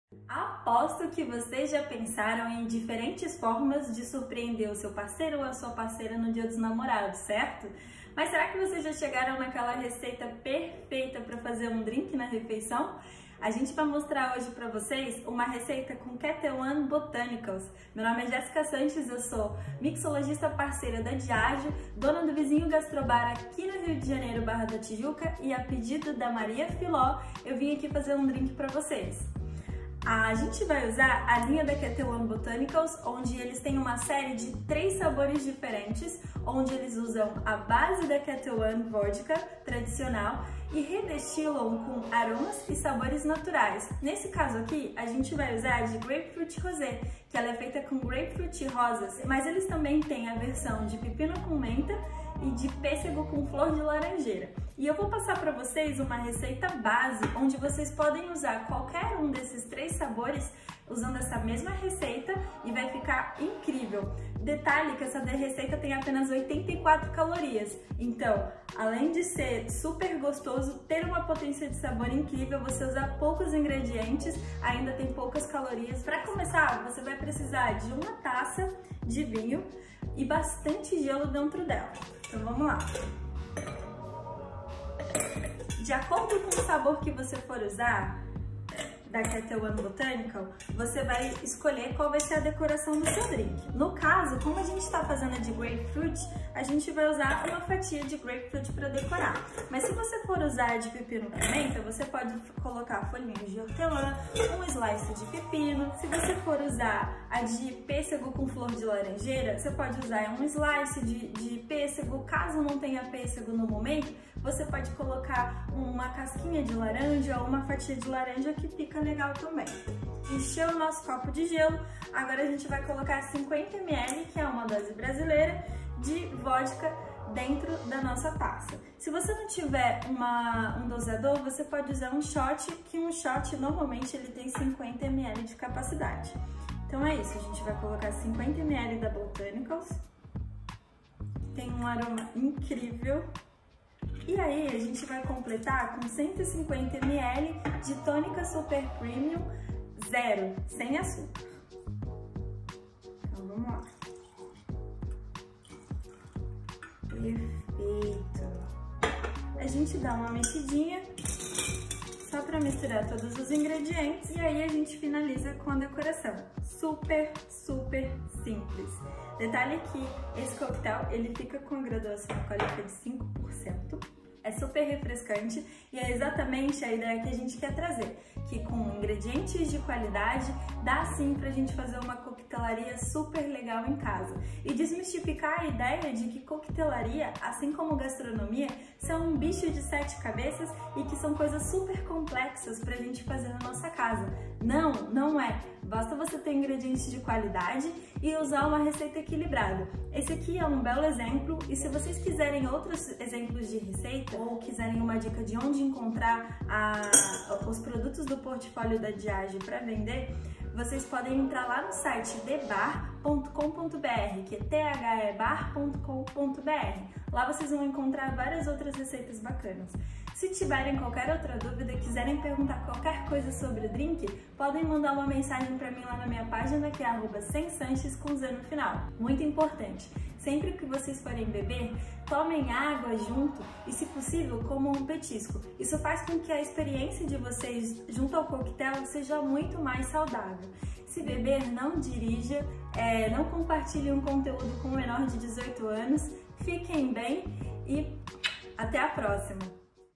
Receita de drink para o Dia dos Namorados
A mixoloigta Jéssica Sanchez ensina a preparar um drink com a vodka @ketelonebotanical, a base de grapefruit e rosas, para você celebrar com seu amor.

Não compartilhe com menores de 18 anos. Beba com moderação.